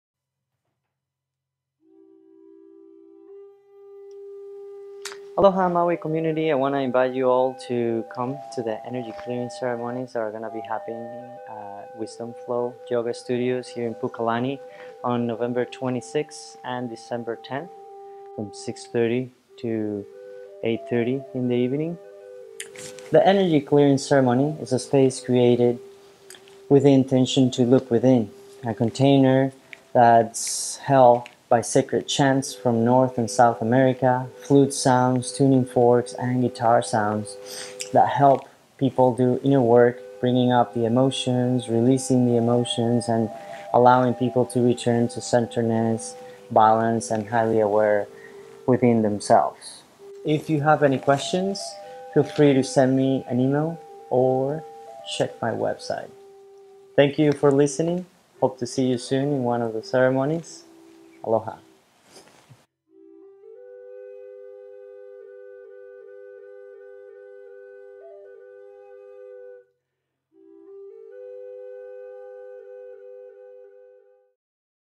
Energy Clearing Ceremonies
Energy Healing Ceremonies at Wisdom Flow Studio on Maui.  Everyone welcome.  From the native traditions of the Amazon these ceremonies use flute, guitar and voice to evoke clearing of the psycho-emotional field to allow a reset and refresh and clearing. See details at WisdomFlowStudio.com